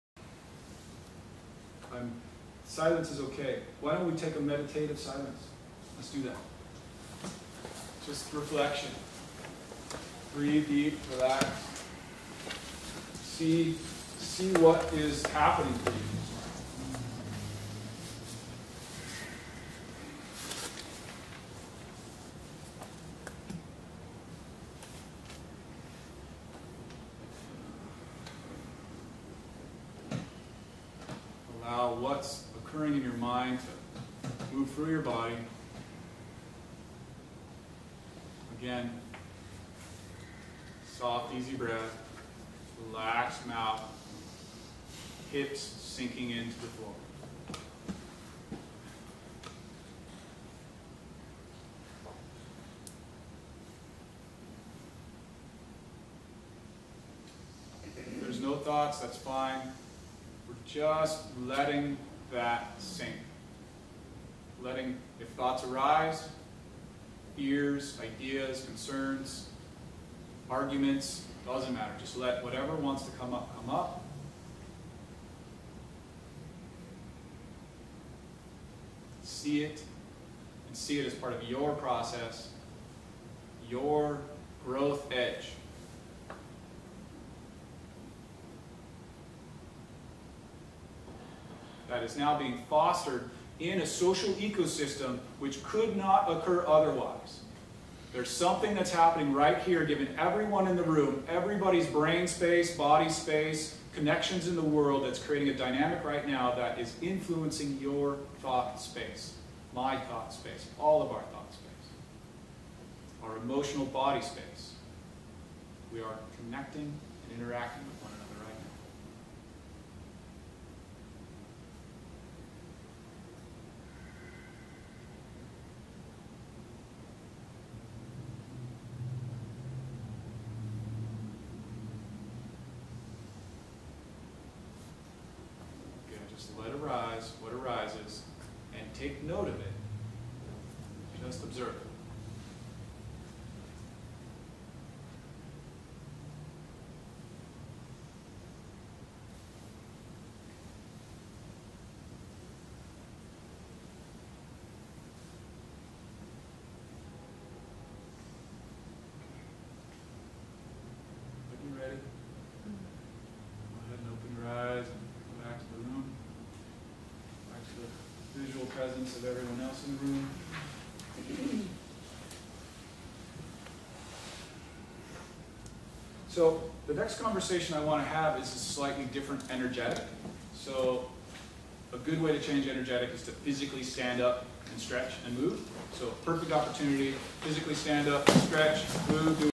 Guided Relaxation Meditation. Observing Thoughts. Connected Pt. 7 EOP
This is a short meditation that is designed to help you process better things you are learning, see your own thought process better, and reinforce the idea that you are constantly being affected by your environment. This segment is from the Ecology of Prosperity Workshop.

✔️✔️ Need Coaching on your project, homestead, or business? Schedule a free 15-minute zoom or phone consultation here.

Relaxation meditation is a valuable skill for the young as stress-based tiredness and anxiety now increasingly affect the younger generation. This video will show you styles you can teach on a one-to-one level or in large groups.

I'm silence is okay why don't we take a meditative silence to let's do that just reflection d for that see what is happening.

Wow, what's occurring in your mind moves through your body again soft easy breath relaxed mouth hips sinking into the floor there are no thoughts that are fine.

We're just letting that sink letting if thoughts arise ears ideas concerns arguments don't matter just let whatever wants to come up see it as part of your process your growth edge that is now being fostered in a social ecosystem that could not occur.

Otherwise, there's something that's happening right here given everyone in the room everybody's brain space body space connections in the world that's creating a dynamic right now that
is influencing your thoughts space my thoughts space all of our thoughts our emotional body space we are connecting an
interactive.

Just let it rise to what arises and take note of it. just observe the visual presence of everyone else. so the next conversation I want to have is about slightly different energy so a good way to change energy is to physically stand up and stretch and move. so perfect opportunity to physically stand up stretch.

I hope that they help you. I want to share everything that I've learned.